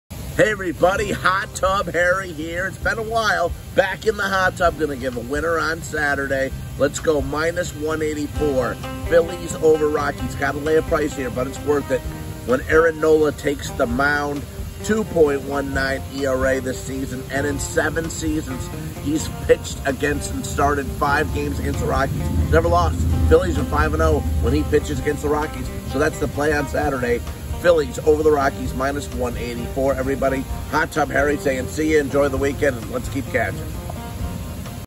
Harry's Pick of the Day | Extra Points - April 24, 2021
Phillies over Rockies -184

Starting pitcher Aaron Nola has never lost a game to the Rockies in his 7 year career. He has faced Colorado 5 times and in all 5 games the Phillies prevailed. Nola has a solid 2.19 ERA and Philadelphia is an overall better team than the struggling Rockies.
7 PHILLIES
4 ROCKIES